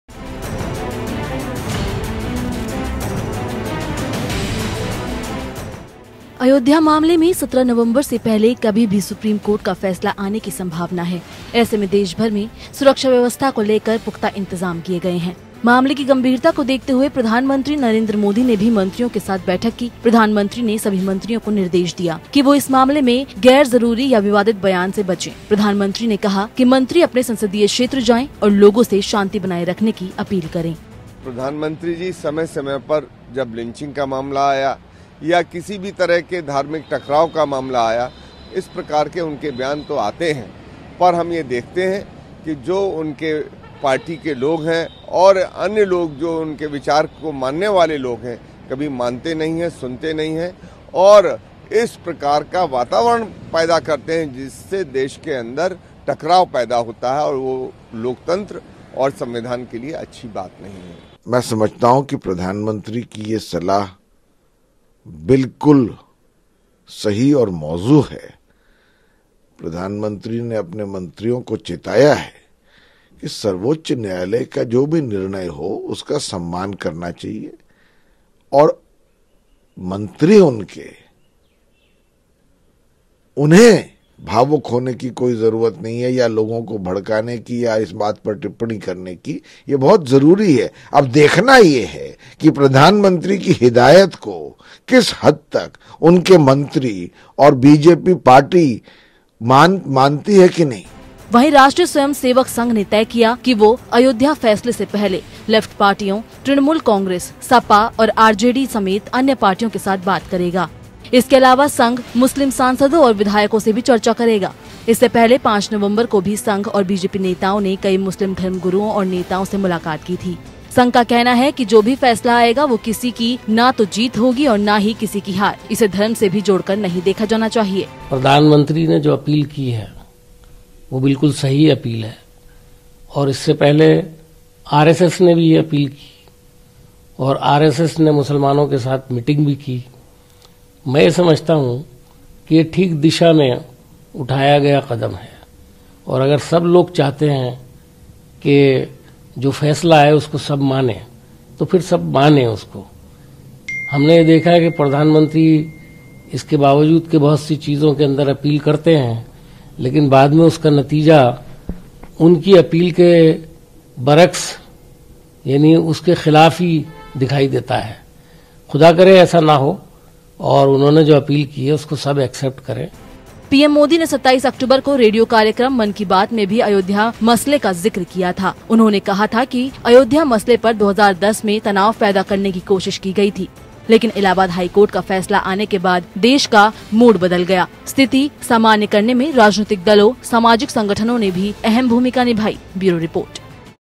अयोध्या विवाद: पीएम मोदी का नेताओं को निर्देश, न करें भड़काऊ बयानबाजी
अयोध्या मामले में 17 नवंबर से पहले कभी भी सुप्रीम कोर्ट का फैसला आने की संभावना है। ऐसे में देशभर में सुरक्षा व्यवस्था को लेकर पुख्ता इंतजाम किए गए हैं। मामले की गंभीरता तो देखते हुए प्रधानमंत्री नरेंद्र मोदी ने भी मंत्रियों के साथ बैठक की। प्रधानमंत्री ने सभी मंत्रियों को निर्देश दिया कि वो इस मामले में गैरजरूरी या विवादित बयान से बचें। पीएम मोदी ने कहा कि मंत्री अपने संसदीय क्षेत्र जाएं और लोगों से शांति बनाए रखने की अपील करें, साथ ही पीएम ने नेताओं से बेवजह भड़काऊ बयानबाजी न करने की अपील भी की है।